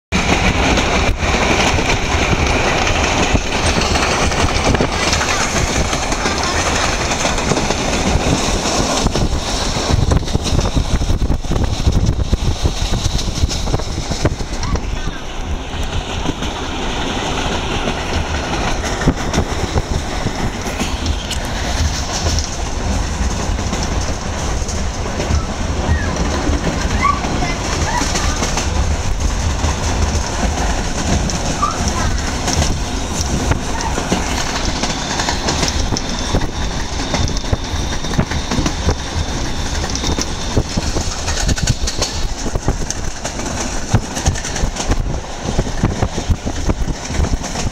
Pociąg 77333 w drodze do Poznania
Pociąg 77333 na czele z Pm36-2 przemierz szlak Wolsztyn - Luboń koło Poznania. 31 lipca 2010.